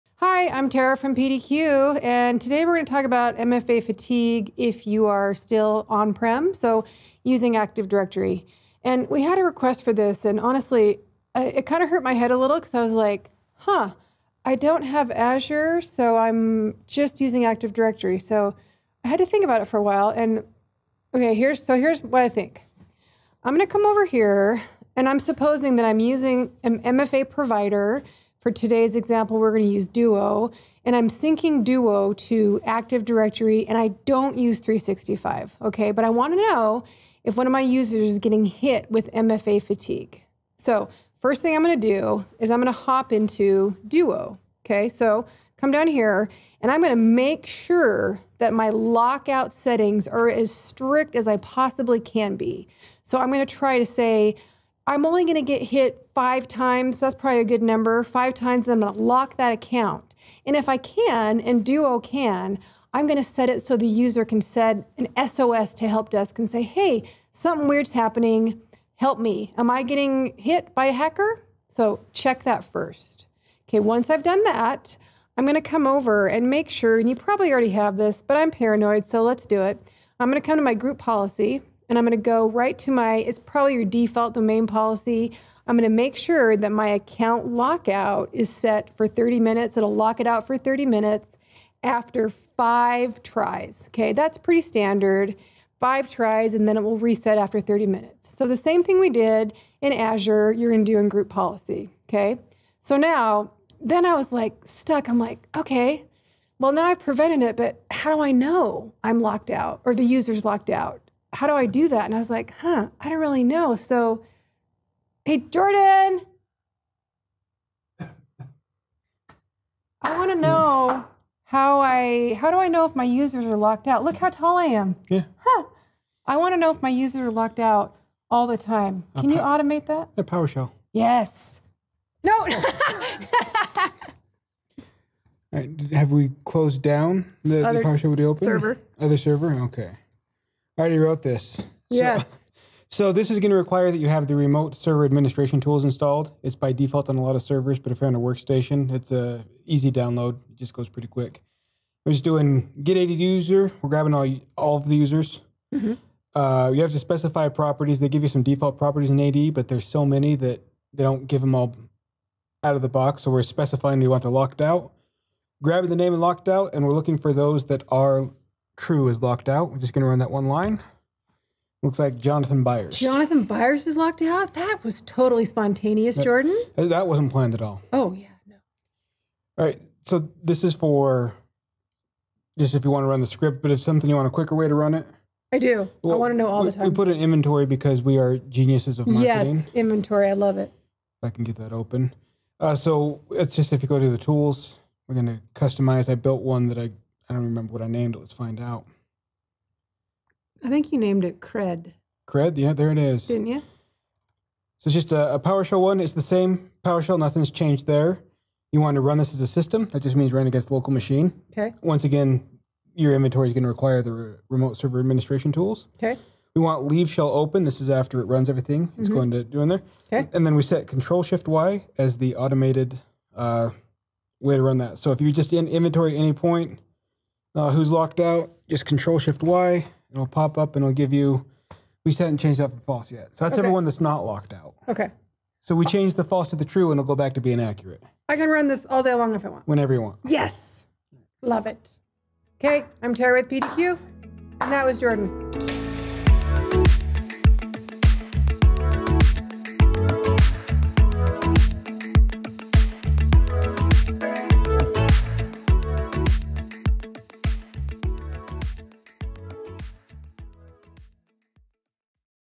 MFA Fatigue Part 2: Using AD with MFA
In a follow-up to the last video, Tara (with Jordan) takes you deeper into the nuances of multi-factor authentication fatigue and how to prevent it from happening to your users. See Part 1 if you missed it